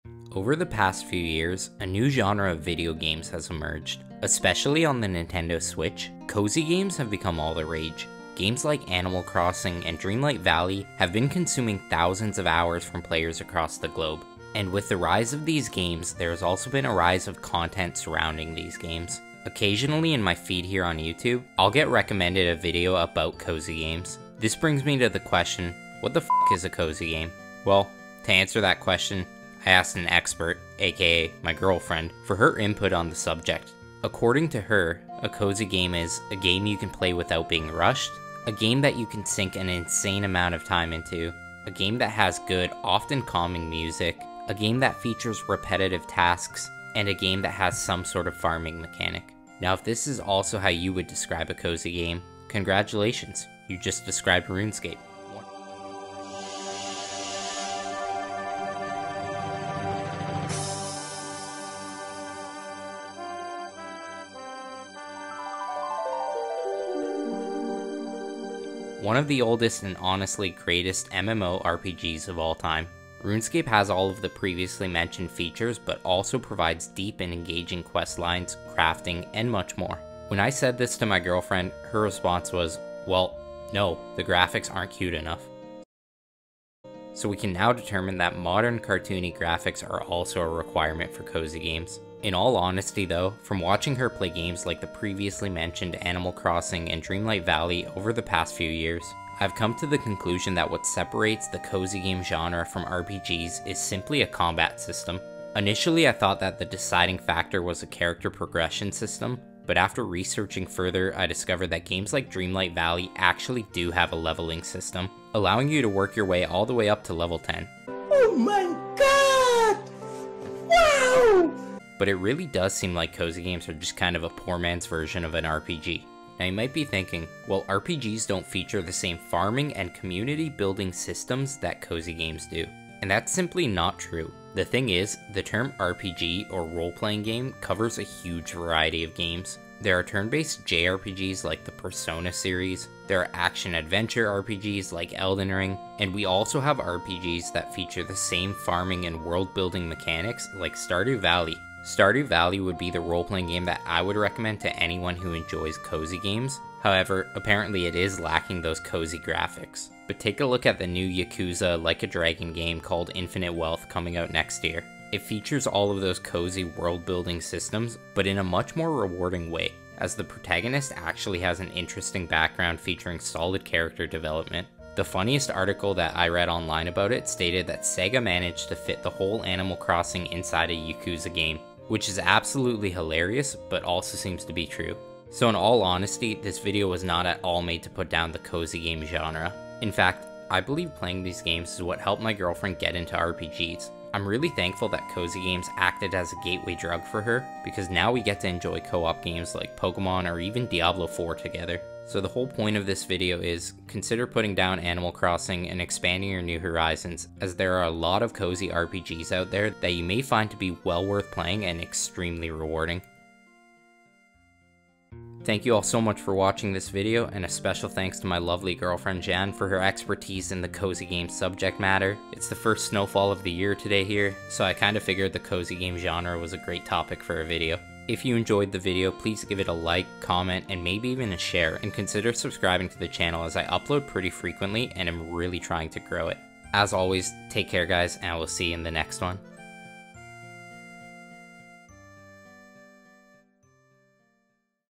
Cozy Games Are Just Poverty RPGs
Cozy Games have grown in popularity recently, especially on the Nintendo Switch. In my opinion, most of these games are just RPGs with less features... But hey, at least the graphics are cute.

Regardless of my opinion, everyone should play video games that they enjoy despite what others think. I really like Diablo 4 and that game gets tons of hate, so ignore the critics.